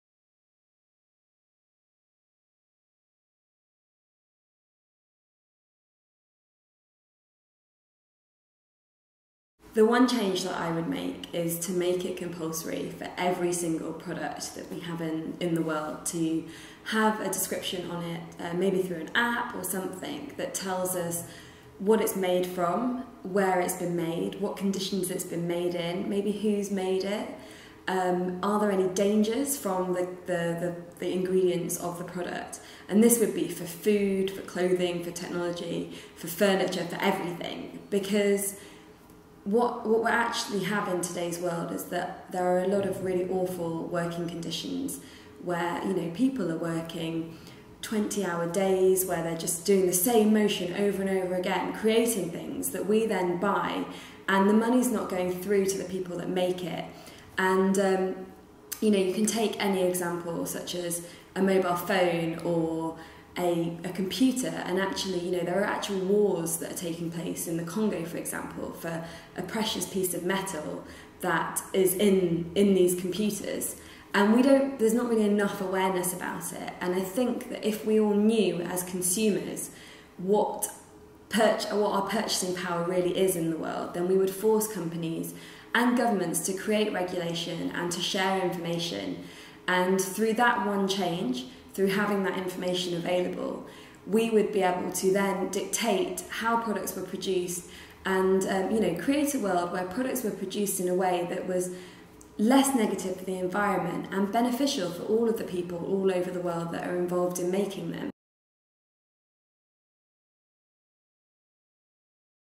Amisha Ghadiali - Product information and consumer power
Amisha Ghadiali is a writer and sustainable fashion campaigner. In her What One Change response, Amisha advocates product information as a means of leveraging consumer power over governments and corporations, creating widespread social change.